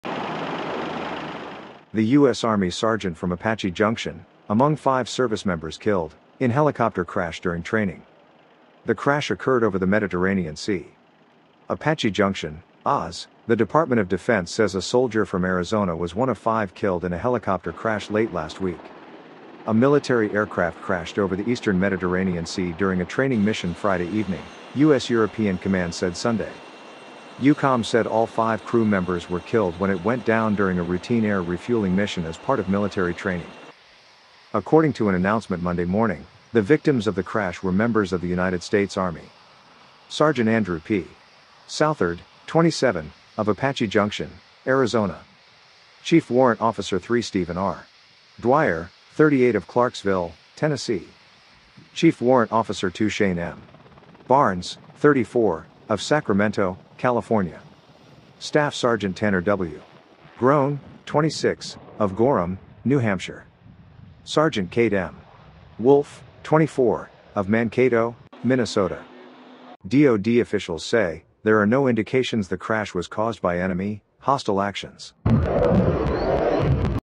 5 U.S. Army Special Operations Aviation Soldiers killed | MH-60 Blackhawk Helicopter Tragedy
"Tragic Loss: Remembering the Fallen Heroes - Helicopter Crash Claims Lives of U.S. Army Special Operations Aviation Soldiers"

"The US Army sergeant from Apache Junction, among five service members killed, in helicopter crash during training"

"The crash occurred over the Mediterranean Sea"

"APACHE JUNCTION, AZ — The Department of Defense says a soldier from Arizona was one of five killed in a helicopter crash late last week."

"A military aircraft crashed over the eastern Mediterranean Sea during a training mission Friday evening, U.S. European Command said Sunday."

"EUCOM said all five crew members were killed when it went down “during a routine air refueling mission as part of military training.”

"According to an announcement Monday morning, the victims of the crash were members of the United States Army:"

"Sgt. Andrew P. Southard, 27, of Apache Junction, Arizona"

"Chief Warrant Officer 3 Stephen R. Dwyer, 38 of Clarksville, Tennessee"

"Chief Warrant Officer 2 Shane M. Barnes, 34, of Sacramento, California"

"Staff Sgt. Tanner W. Grone, 26, of Gorham, New Hampshire"

"Sgt. Cade M. Wolfe, 24, of Mankato, Minnesota"

"DOD officials say," "there are no indications the crash was caused by enemy / hostile actions."

The Department of Defense announced today the deaths of five U.S. Army Special Operations aviation Soldiers killed during routine flight training as a result of a helicopter crash, Nov. 10, 2023 in the Mediterranean Sea. There are no indications the crash was caused by enemy / hostile actions.

The MH-60 Blackhawk was conducting aerial refueling training when the aircraft experienced an in-flight emergency, resulting in the crash.

Our thoughts and prayers remain with the families of the fallen. The U.S. Army's Combat Readiness Center is conducting an investigation into the incident.